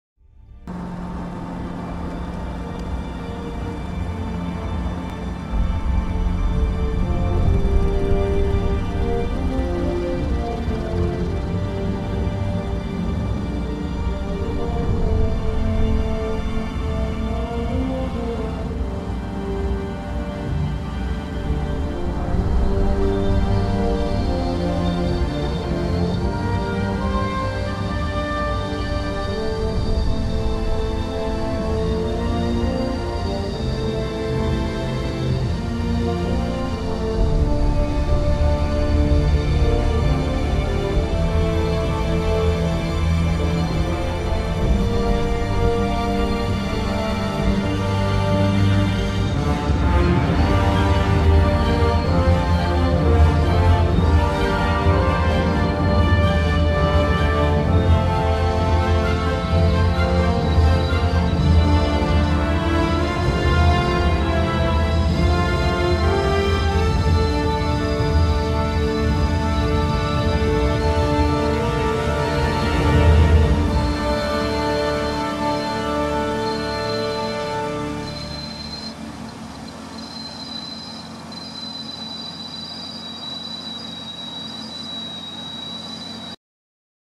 New zealand Fiordland Helicopter Fligh
Traversée en hélicoptère du Parc National du Fiordland en Nouvelle Zélande